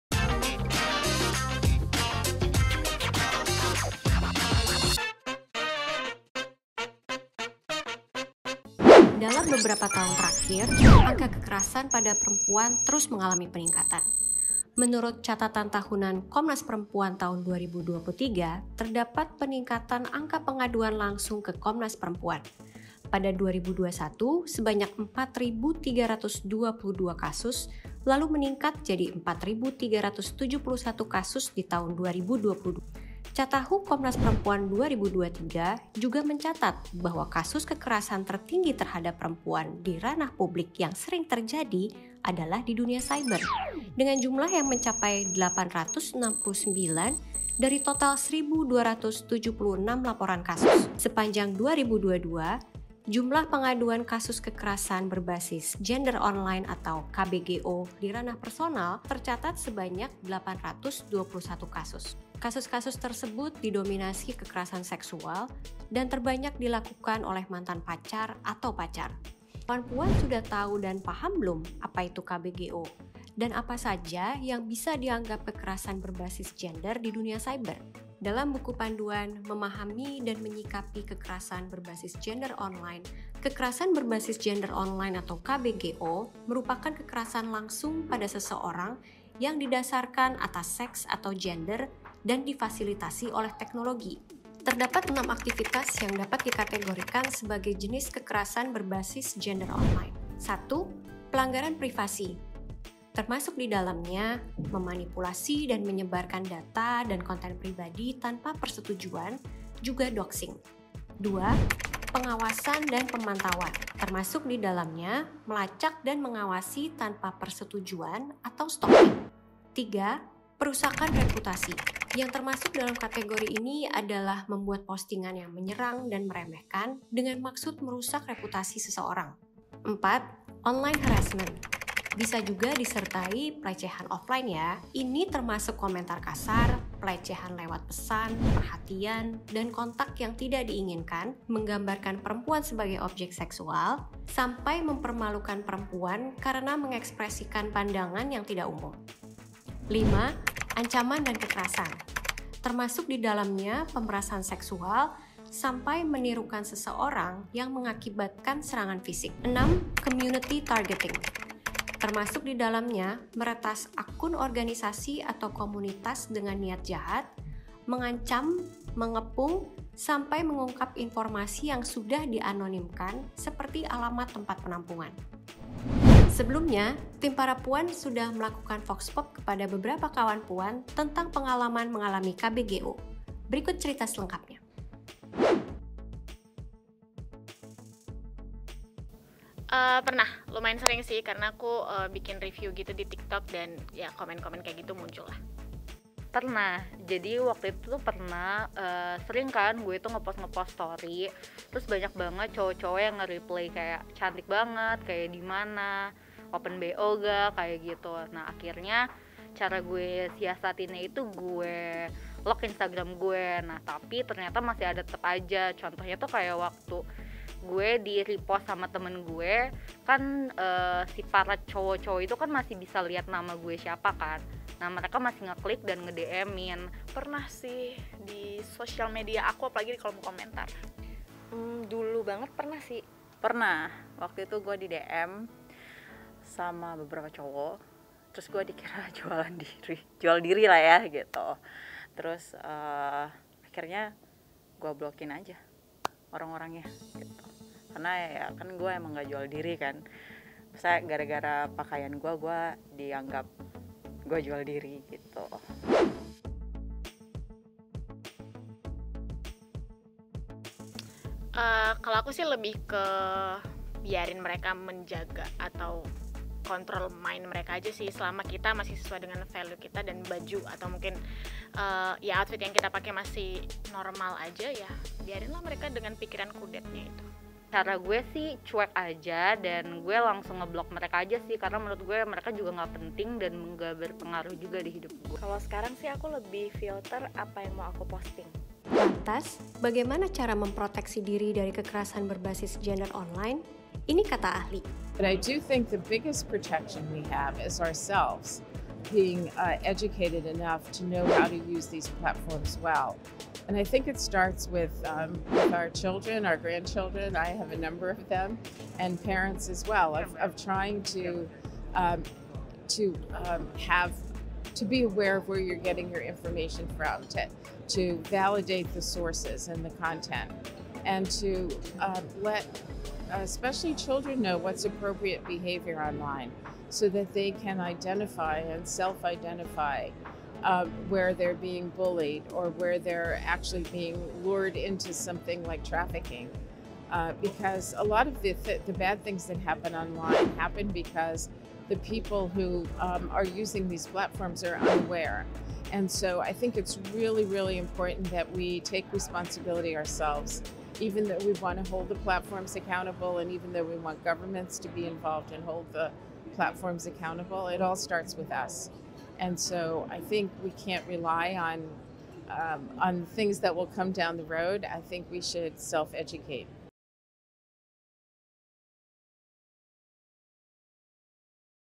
Pengalaman Kawan Puan Alami KBGO dan Tips Cara Melindungi Diri dari KBGO | Trenpuan Episode 48
Ternyata masih banyak loh Kawan Puan yang pernah mengalami KBGO. Apa sih KBGO? Untuk lebih jelasnya yuk saksikan Trenpuan episode kali ini yaa!

Happy Watching!